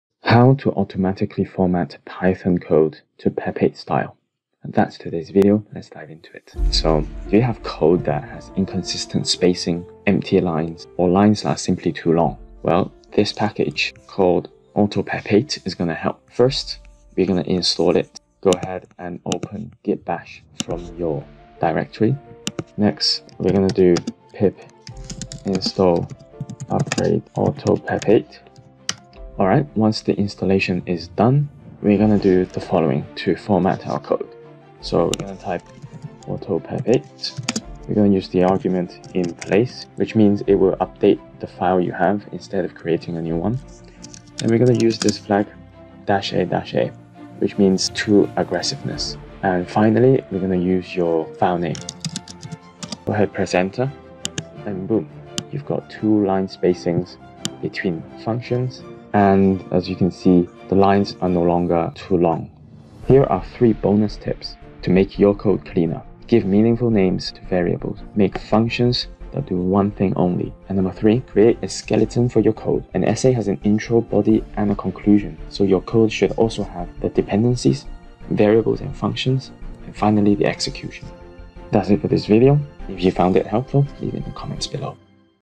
HOW TO: Automatically Format Python Code to PEP8 Style? (+ BONUS Tips):
Autoformat: autopep8 --in-place -a -a file.py

3 Bonus tips for clean code:
• Give meaningful names to variables.
• Make functions that do one thing only.
• Create a skeleton for your code. An essay has an intro, a body, and a conclusion. In code, you have your dependencies, variables and functions, and then execution.